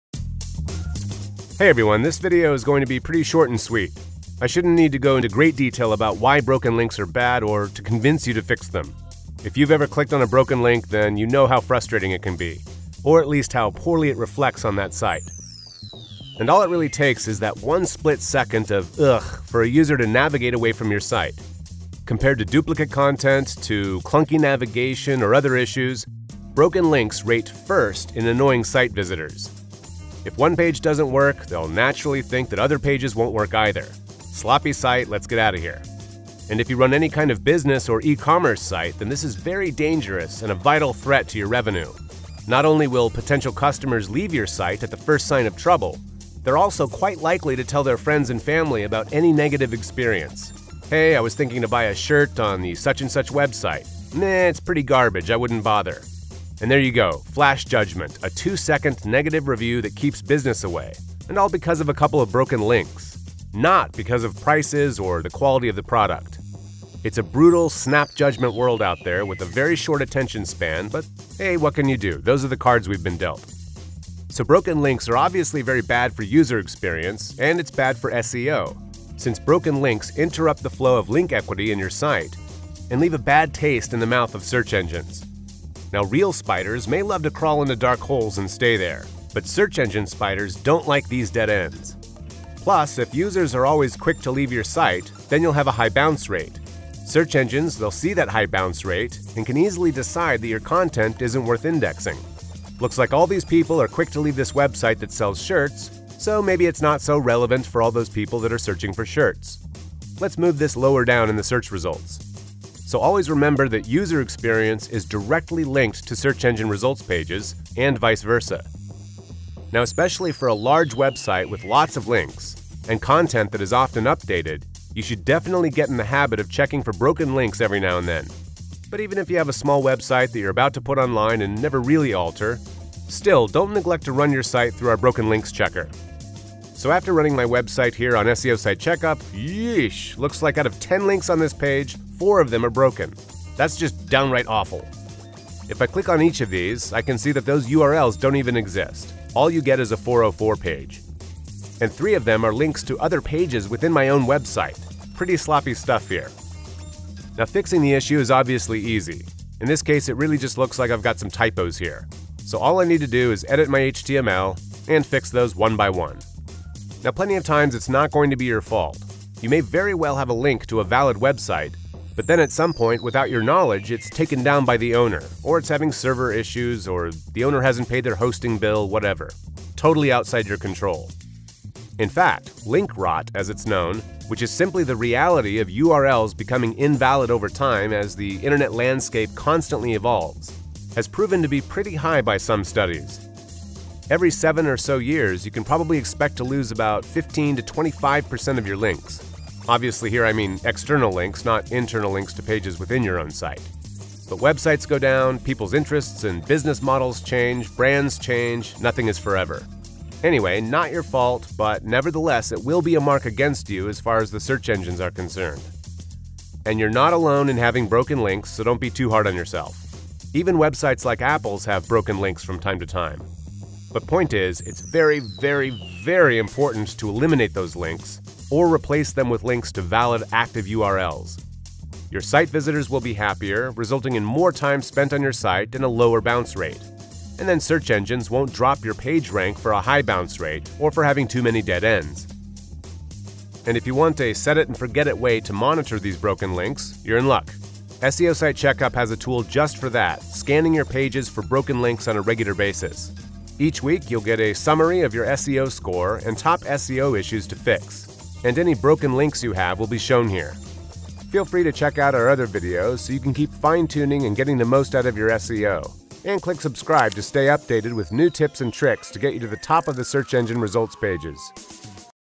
How to Find and Fix Broken Links
In this video, you’ll hear some of the following:

"If you've ever clicked on a broken link then you know how frustrating it can be or at least how poorly it reflects on that site and all it really takes is that one split second of look for a user to navigate away from your site.

Compared to duplicate content to clunky navigation or other issues broken links rate first in annoying site visitors. If one page doesn't work they'll naturally think that other pages won't work either sloppy site let's get out of here.

And if you run any kind of business or e-commerce site then this is very dangerous and a vital threat to your revenue not only will potential customers leave your site at the first sign of trouble they're also quite likely to tell their friends and family about any negative experience.

Hey I was thinking to buy a shirt on these such-and-such website man it's pretty garbage I wouldn't bother and there you go flash judgment a two seconds negative review that keeps business away and all because of a couple of broken links not because of prices or the quality of the product it's a brutal snap judgment world out there with a very short attention span but hey what can you do those are the cards we've been dealt.

So broken links are obviously very bad for user experience and it's bad for SEO."

Hear more by watching the full video!